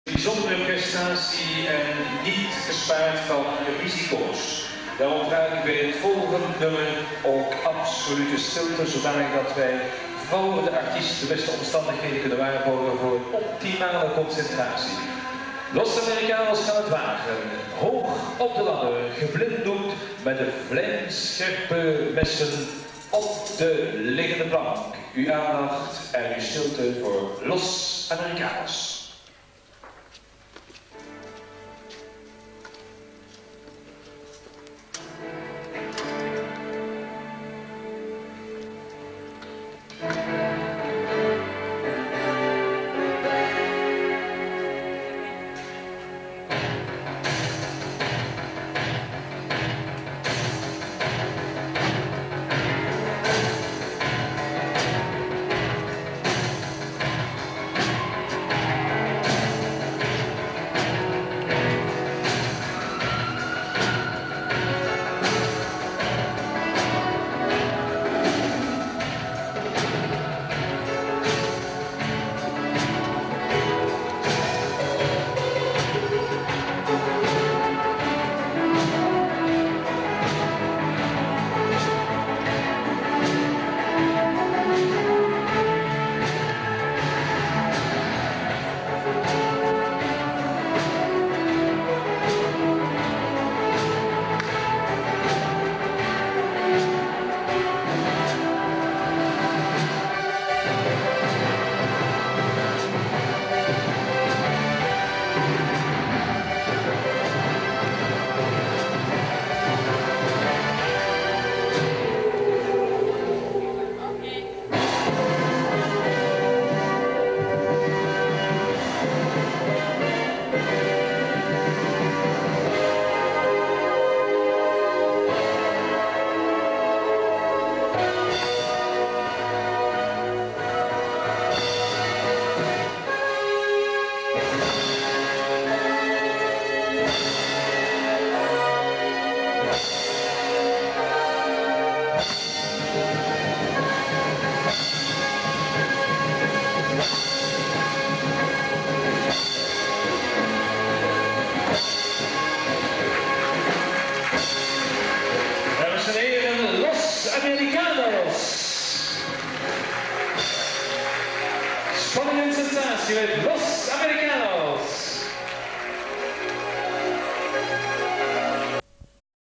Kerstcircus Hasselt 1988
Kerstcircus vond plaats te Hasselt in 1988 in het Cultureel Centrum,grote schouwburg.Er werd deelgenomen door alle Belgische circussen.Ons optreden vond plaats onder de naam : " Los Americanos" en was de eerste act ter wereld met Ninja stars.